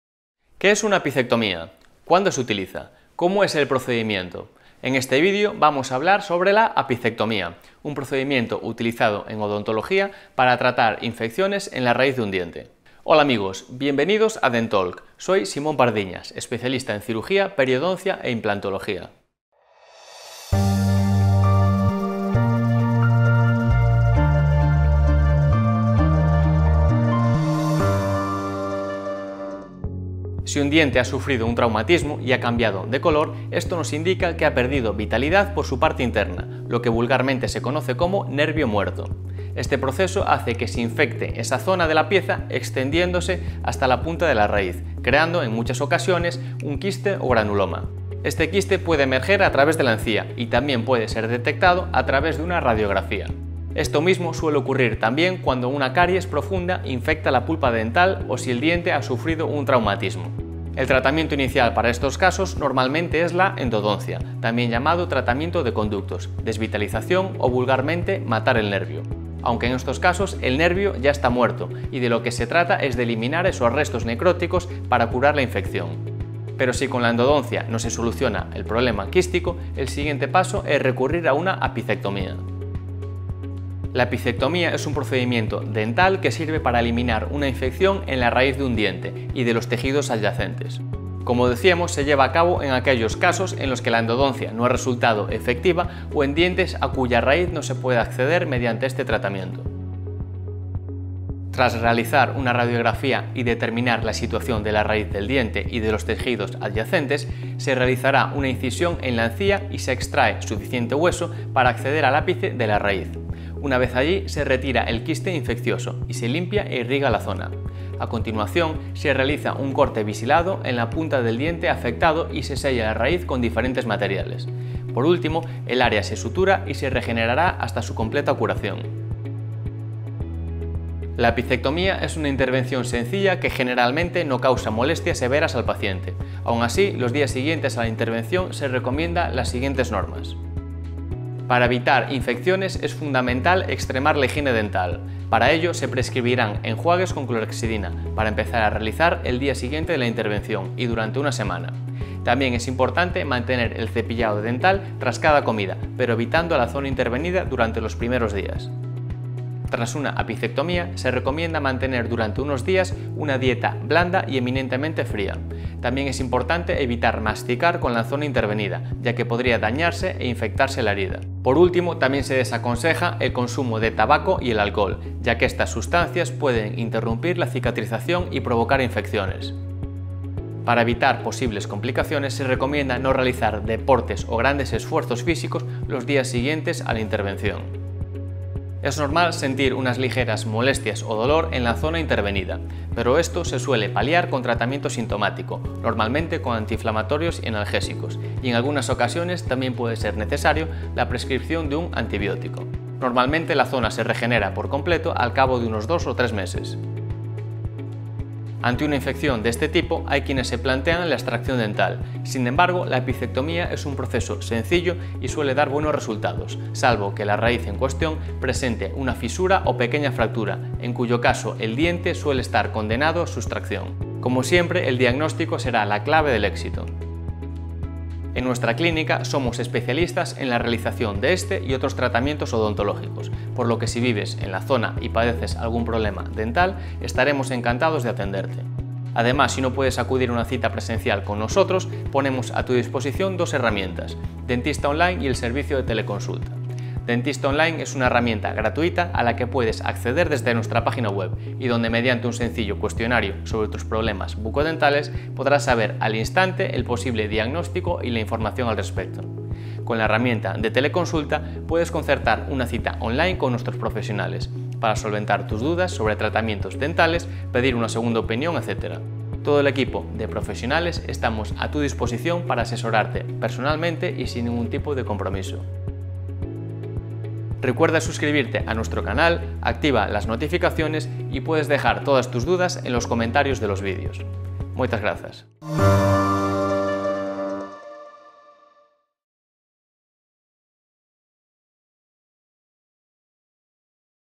APICECTOMÍA - Así se elimina una INFECCIÓN en la raíz del DIENTE | Dentalk! ©
¿Qué es una apicectomía? ¿Cuándo se utiliza? ¿Cómo es el procedimiento? Aquí hablamos sobre la apicectomía, un procedimiento utilizado en odontología para tratar infecciones en la raíz del diente no abordables con una endodoncia.

0) Introducción 00:00.
Hablamos sobre la apicectomía, su realización y su posoperatorio en este Dentalk de Clínica Médico Dental Pardiñas

1) Cuándo realizar una apicectomía 00:30.
Si un diente ha sufrido un traumatismo y ha cambiado de color, esto nos indica que ha perdido vitalidad por su parte interna, lo que vulgarmente se conoce como “nervio” muerto. Este proceso hace que se infecte esa zona de la pieza extendiéndose hasta la punta de la raíz, creando, en muchas ocasiones, un quiste o granuloma. El tratamiento inicial para estos casos es la endodoncia, pero si esto no soluciona el proceso quístico, el siguiente paso es la apicectomía.

2) ¿Qué es una apicectomía? 01:24.
La apicectomía es un procedimiento odontológico que sirve para eliminar una infección en la raíz de un diente y de los tejidos adyacentes. Como decíamos, se lleva a cabo en aquellos casos en los que la endodoncia no ha resultado efectiva o en dientes a cuya raíz no se puede acceder mediante ese tratamiento.

3) ¿Cómo se realiza una apicectomía? 01:41.
Tras realizar una radiografía y determinar la situación de la raíz del diente y los tejidos adyacentes, se realizará una incisión en la encía y se extrae suficiente hueso para acceder al ápice de la raíz. Una vez allí, se retira el quiste infeccioso y se limpia e irriga la zona. A continuación, se realiza un corte biselado en la punta del diente afectado y se sella la raíz con diferentes materiales. Por último, el área se sutura y se regenerará hasta su completa curación.

4) ¿Cómo es el posoperatorio de la apicectomía? 02:10.
La apicectomía es una intervención sencilla que generalmente no causa molestias severas al paciente. Aún así, los días siguientes a la intervención se recomienda mantener la zona correctamente higienizada, mantener una dieta adecuada, controlar la actividad física y tomar la medicación recomendada.

5) ¿Apicectomía o extracción dental? 03:28.
Ante una infección de este tipo, hay quienes se plantean la extracción dental. Sin embargo, la apicectomía es un proceso sencillo y suele dar buenos resultados, salvo que la raíz en cuestión presente una fisura o pequeña fractura, en cuyo caso el diente suele estar condenado a su extracción. Como siempre, el diagnóstico es la clave del éxito.

6) ¿Podemos ayudarte? 03:48.
En Clínica Pardiñas somos especialistas en la realización de este y otros tratamientos odontológicos en A Coruña. Si no eres de aquí, puedes solicitar una consulta online con nuestro equipo de doctores